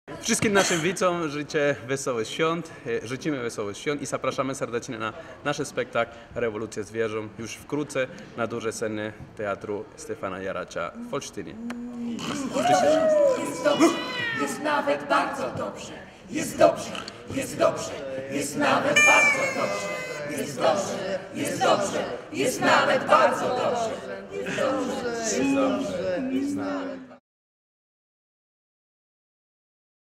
"Rewolucja zwierząt" życzenia świąteczne
Od 30 grudnia zapraszamy na Scenę Dużą na spektakl "Rewolucja zwierząt".

Minęło ileś lat od przewrotu w folwarku zwierzęcym – zwierzęta pod rządami Wieprza Alfa wiodą spokojne, pracowite życie – spichlerz jest do połowy pełny, prowadzą swoje gospodarstwo bez udziału ludzi, za to współpracują z ludźmi w kwestii maszyn i innych rzeczy, których same nie potrafią wyprodukować. Wieprz Alfa jest przekonany, że kolejny raz wygra wybory, bo zapewnił mieszkańcom ciepłą wodę w kranie.

Jednak pojawia się inny kandydat – Wieprz z zakrzywionym ogonkiem – który mówi zwierzętom, że spichlerz nie jest do połowy pełny, tylko do połowy pusty, że zwierzęta nie są wolne, bo ciągle współpracują z ludźmi, że nadal nie rozliczono poprzedniej rewolucji i straszy zwierzęta różnymi  zagrożeniami. Obiecuje też zwierzętom, że jeśli go wybiorą, będą mogły jeść tyle, ile będą chciały, a psom obiecuje to, co mają plus to, co będą chciały. Dzięki obietnicom i eskalacji strachu wygrywa wybory. Nawet rozsądny głos osła, który pamięta poprzednią rewolucję zwierząt nic nie daje, ponieważ zostaje zakneblowany na rozkaz wieprza z zakrzywionym ogonkiem.

Nowy wódz zrywa kontakty z ludźmi, nie chce dać schronienia bezbronnym zwierzętom leśnym, które uciekają przed pożarem i lekceważy Białego Kruka apelującego o pokój i pomoc potrzebującym.

Po fazie rozdawania jedzenia nadchodzi jednak czas prawdziwych problemów z zapewnieniem bezpieczeństwa i żywności...